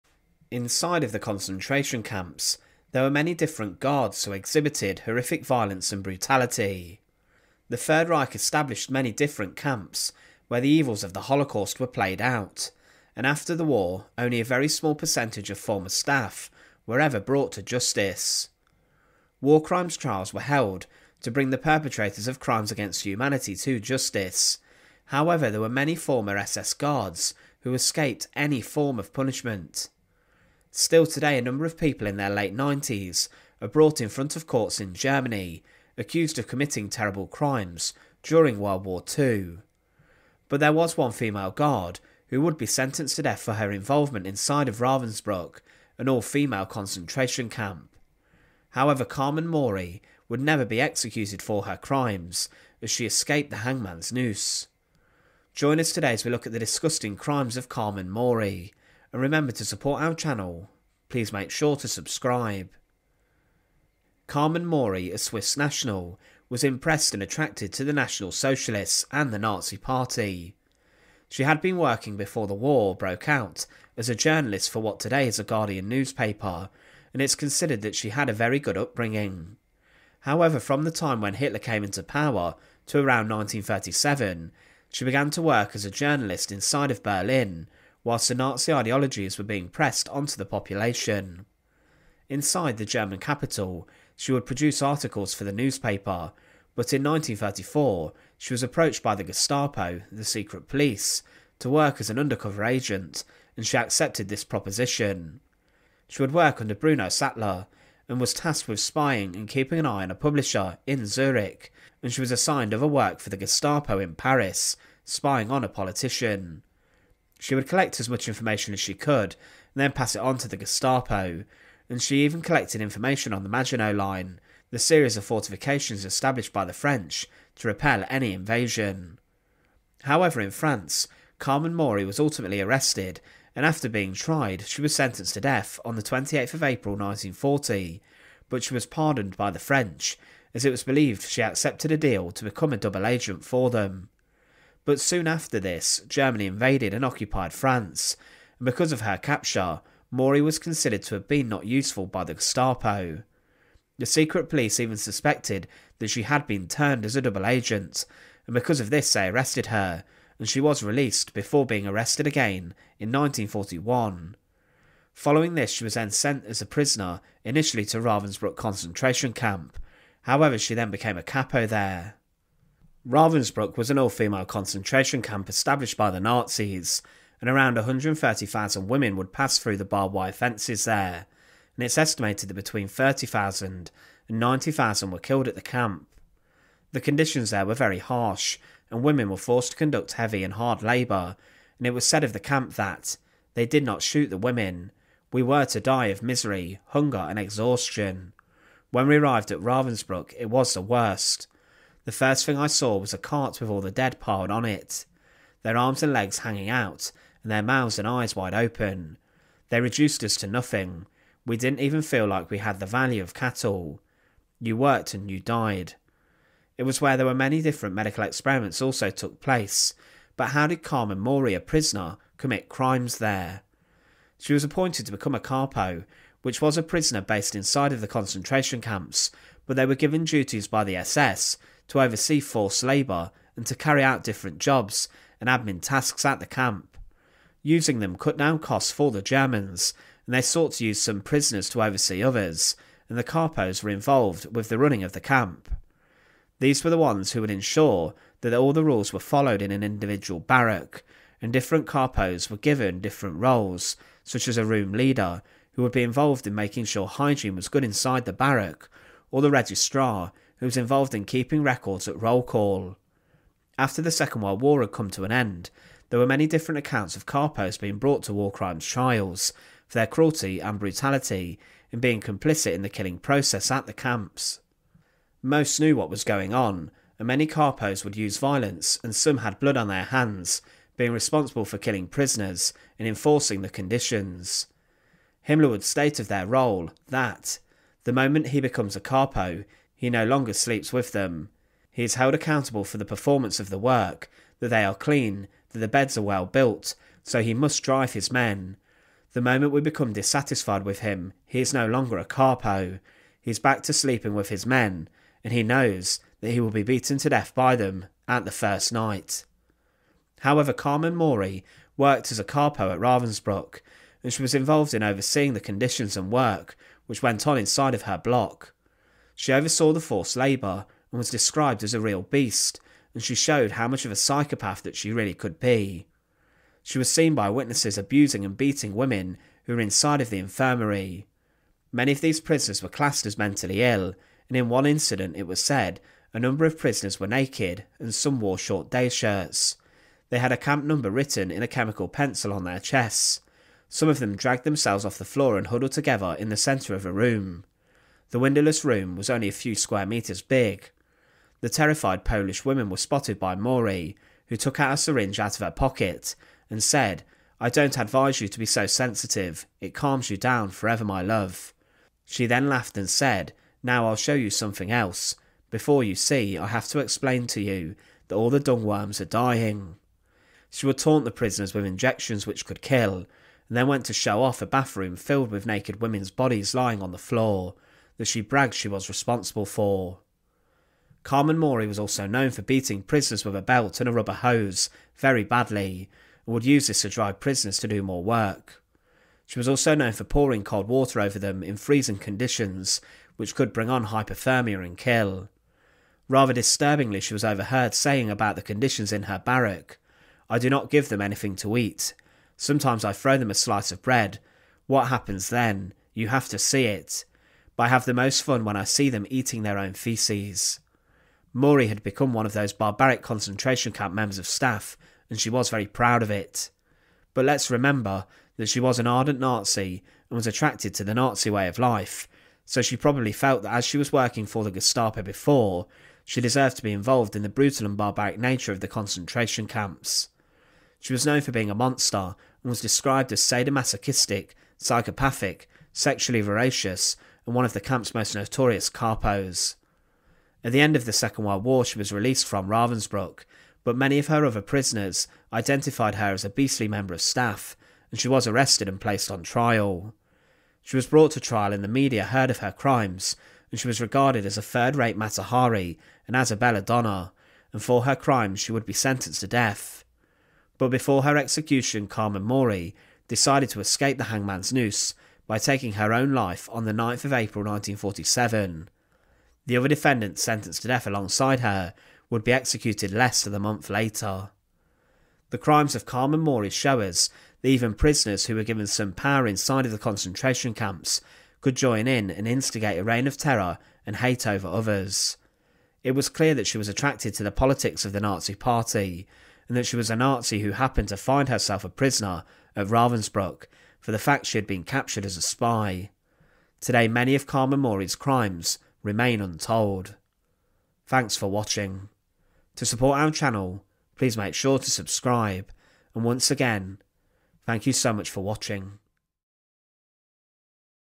The DISGUSTING Crimes Of Carmen Mory - The Black Angel Of Ravensbruck
During the Second World War, there were a number of female concentration camp guards who inflicted such suffering onto the prisoners of camps such as Bergen-Belsen and Auschwitz. There was an all-female concentration camp established within the Third Reich known as Ravensbruck, and inside of here thousands of women were kept in terrible conditions. The SS used many prisoners inside the camp to complete different roles and oversee barracks and these were known as Kapos. A Kapo was usually hated by the other prisoners, and many administered violence and beatings on the prisoners almost like guards would do, and a number were sentenced to death and executed after the end of World War Two. One such Kapo who worked at Ravensbruck was Carmen Mory.

Carmen Mory actually was a spy who worked for the Gestapo, but after being captured by the French and released, the Gestapo sent her to Ravensbruck as a prisoner fearing she was a double agent. She became a Kapo and was known as 'The Black Angel,' for her violence and beatings of prisoners. She was involved in the killing process, and inside the camp would refuse to feed the prisoners of her barrack and would take joy in watching them starve. She forced them to work incredibly hard, but was later tried for her crimes and was sentenced to death. However she would never encounter the hangman's noose...

Join us today as we look at, 'The DISGUSTNG Crimes Of Carmen Mory - The Black Angel of Ravensbruck.'